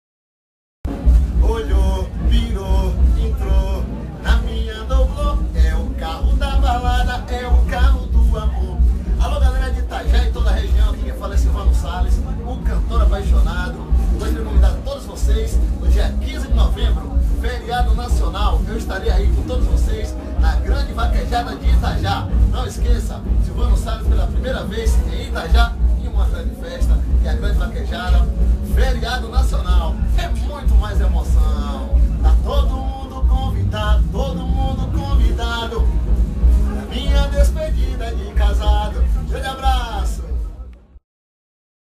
SILVANNO SALLES CONVIDA A TODOS P/ VAQUEJADA DE ITAJÁ-RN
A MAIOR VAQUEJADA DO INTERIOR DO RN VEM MAIS UMA VEZ COM GRANDES ATRAÇÕES!
SEXTA 15 NOVEMBRO: SILVANNO SALLES + FORRÓ DA RAPAZIADA + DEIXE CUM NOIS
SÁBADO 16 NOVEMBRO: FORRÓ DA PEGAÇÃO + SIRANO & SIRINO + ZÉ SANFONEIRO

REALIZAÇÃO: GRUPO J LOPES